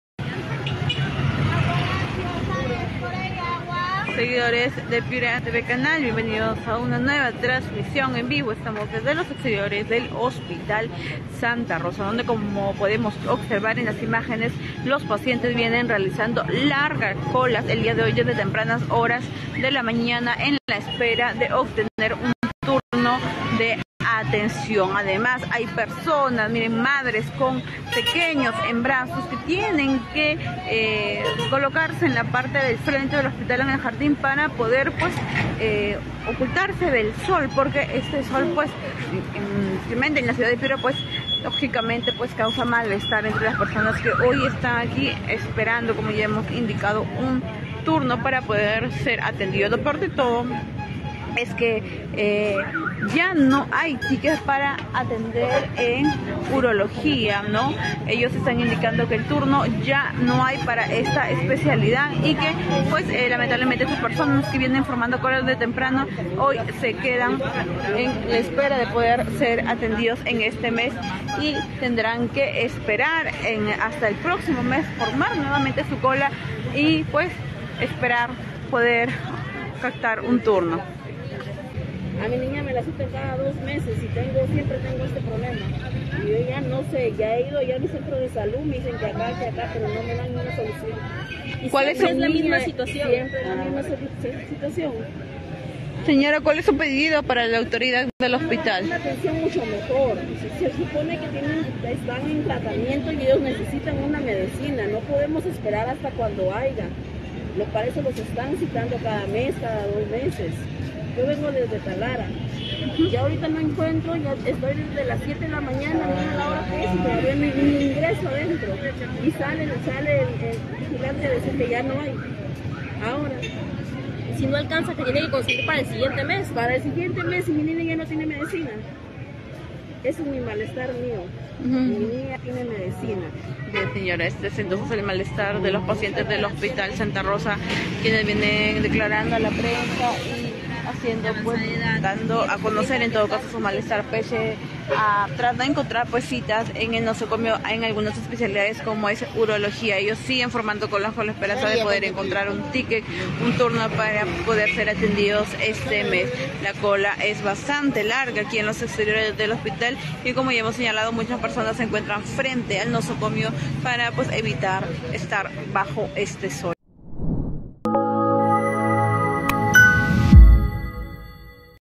26deoctubre | Pacientes del hospital Santa Rosa forman largas colas para ser atendidos en Piura
Bajo el inclemente sol y cubriéndose con lo que pueden, pacientes del hospital Santa Rosa forman largas colas con la esperanza de lograr ser atendidos.

Esta es la situación en las afueras de dicho nosocomio y el clamor popular es uno: ser atendidos debido a que muchos están esperando desde la madrugada y han tenido que viajar desde otras ciudades de la región.